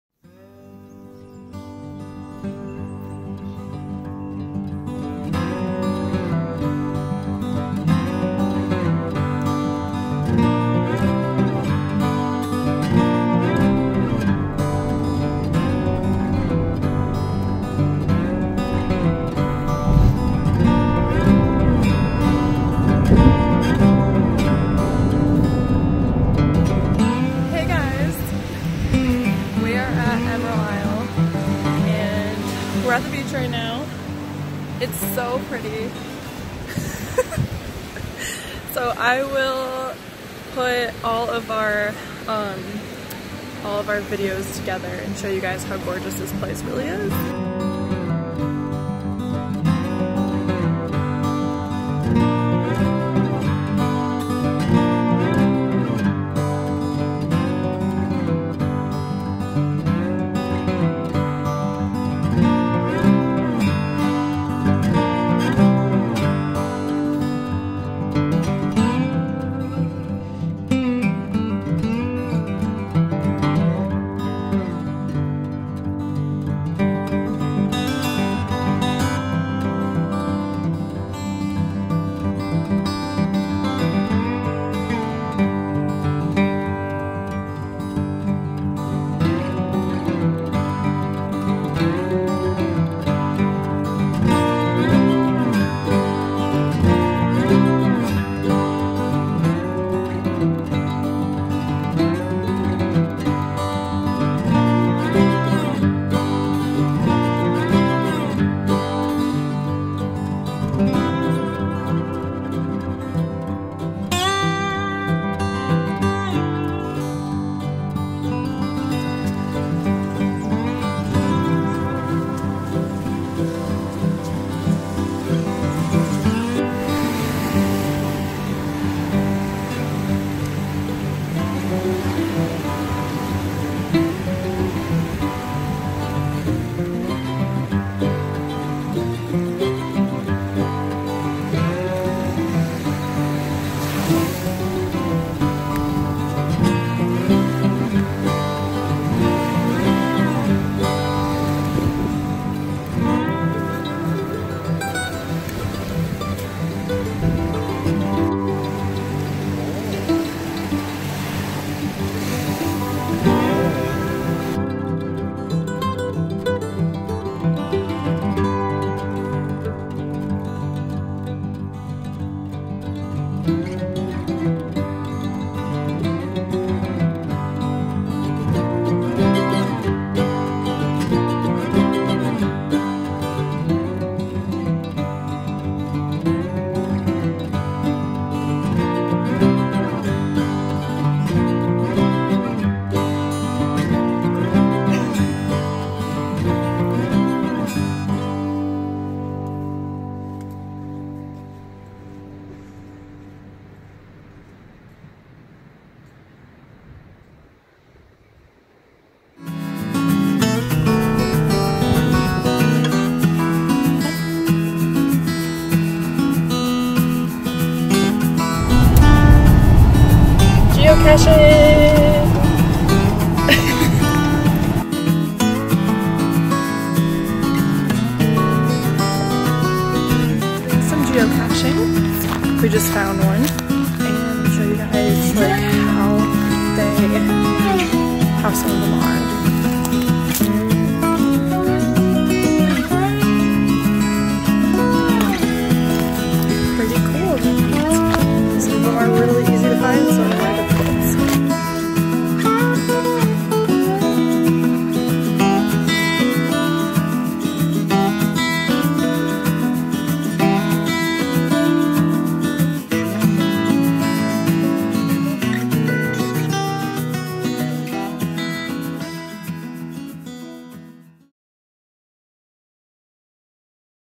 Emerald Isle Vlog NC
Emerald Isle is really close to us and before we leave NC we wanted to remember how gorgeous this place is!